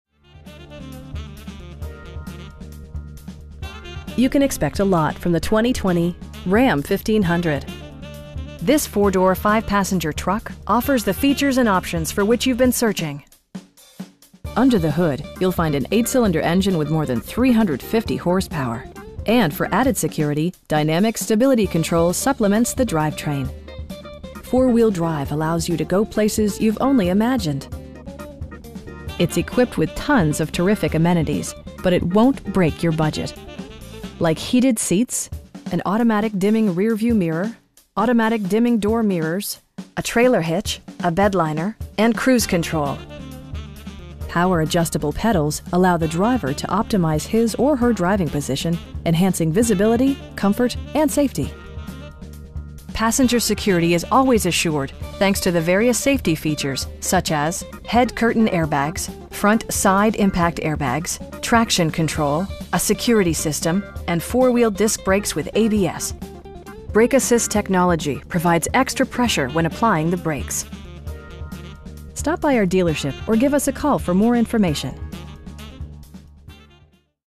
2020 Ram 1500 BIG HORN CREW CAB 4X4 5'7 BOX
Prime Chrysler Dodge Jeep Ram
824 Portland Rd, Route 1
in Saco, ME 04072

You can expect a lot from the 2020 Ram 1500 This 4 door, 5 passenger truck offers the features and options for which you've been searching. Under the hood you'll find an 8 cylinder engine with more than 350 horsepower, and for added security, dynamic Stability Control supplements the drivetrain. Four wheel drive allows you to go places you've only imagined. It's equipped with tons of terrific amenities, but it won't break your budget. Like heated seats, an automatic dimming rear-view mirror, automatic dimming door mirrors, front bucket seats, an overhead console, a trailer hitch, a bedliner, and cruise control. Power adjustable pedals allow the driver to optimize his or her driving position, enhancing visibility, comfort and safety. Passenger security is always assured thanks to various safety features, such as: head curtain airbags, front side impact airbags, traction control, a security system, and 4 wheel disc brakes with ABS. Brake assist technology provides extra pressure when applying the brakes. We pride ourselves on providing excellent customer service. Stop by our dealership or give us a call for more information.